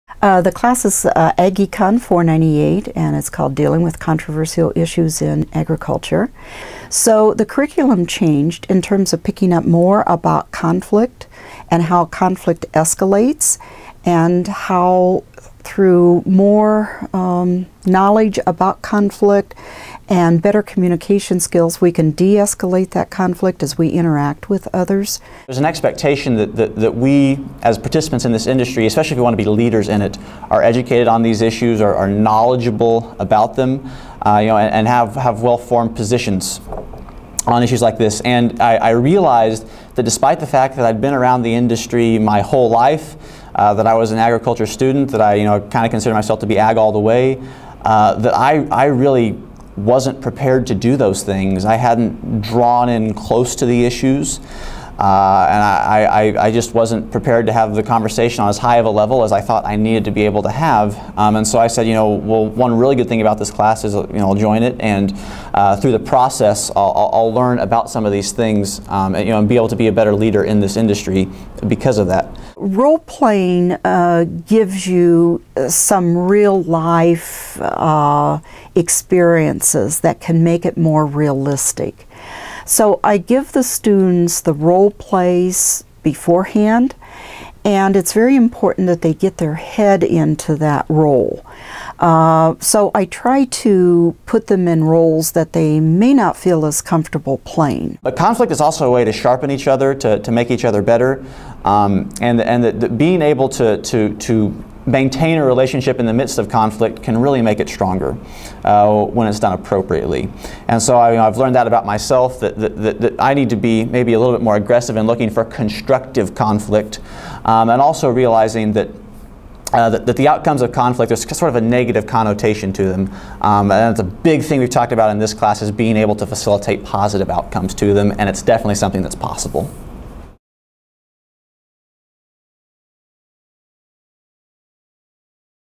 Controversial Issues In Agriculture
Purdue University course teaches students how to help resolve issues in agriculture through leadership and collaboration.